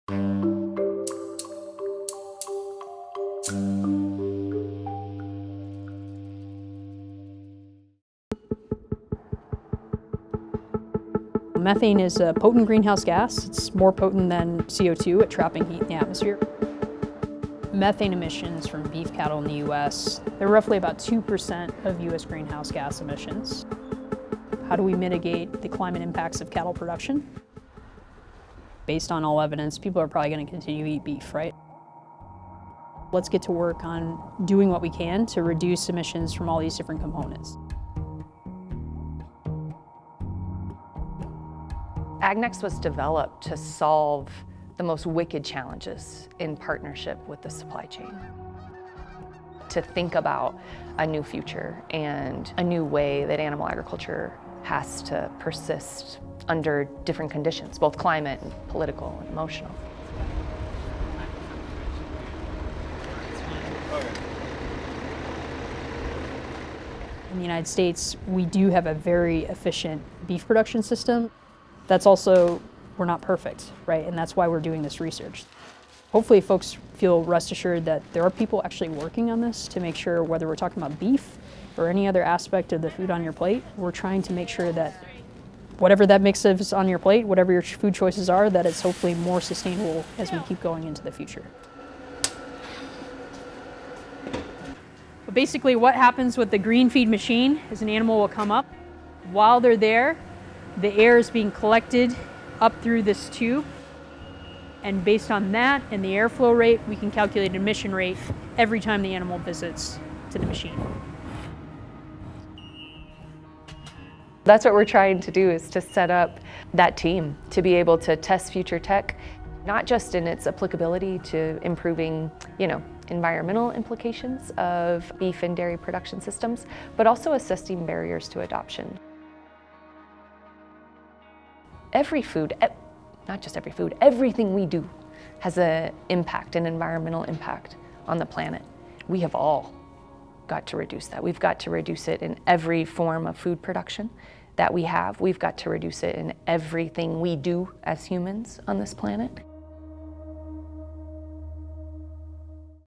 Testing new feed that reduces methane from burping cows | The Protein Problem
Cattle belch out serious amounts of greenhouse gases, especially methane — about 220 pounds a year of methane, which is 80 times more harmful than carbon dioxide, the main greenhouse gas spewed out by cars.

A new group called AgNext, led by Colorado State University, is delving into sustainability issues at a small feedlot near campus. AgNext receives funding from the beef industry, and researchers say they hope to work closely with ranchers and other producers to implement solutions.